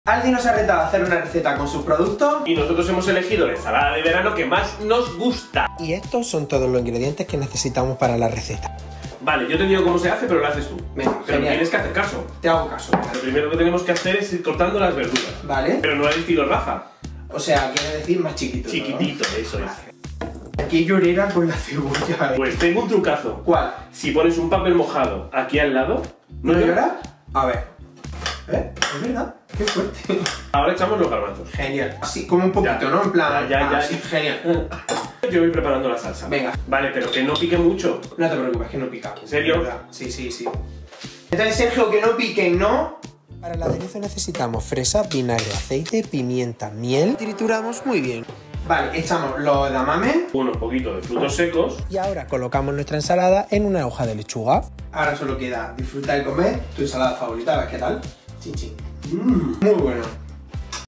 Ensalada de verano
Hablando de raciones nos prepara su versión favorita de una ensalada de verano con los garbanzos de El Cultivador. Fácil, fresca y riquísima. ​

Te dejamos aquí los ingredientes que necesitas para prepararla: ​
- Garbanzos​
- Edamames​
- Tomate​
- Pimientos verde y rojo​
- Cebolla ​
- Frutos secos​

Para la vinagreta:​
- Fresa​
- Vinagre​
- Sal​
- Pimienta ​
- Miel​